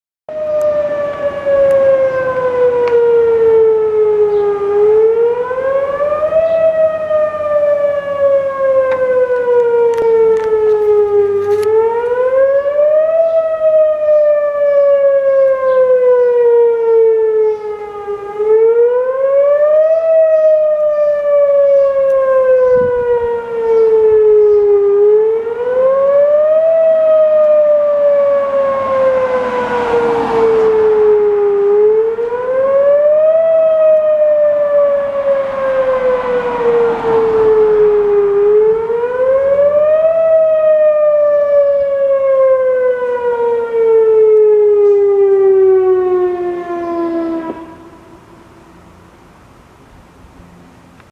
Cranbury VFC Whelen WPS-2902 Fire Siren 7/13/22
Sounding for a call (alarm activation) in Cranbury, NJ on the afternoon of Wednesday July 13th, 2022 in Middlesex County. This Whelen WPS-2902 siren located at the fire station suddenly went off just as I was coming down South Main Street & it caught me completely by surprise so I missed the first two cycles of Wail but was still able to catch the response minutes later however after parking the car. I hope to catch more of these things soon & was so glad that I turned left at the light 🚦 earlier or else I would've missed out completely. Apologies for the shakiness.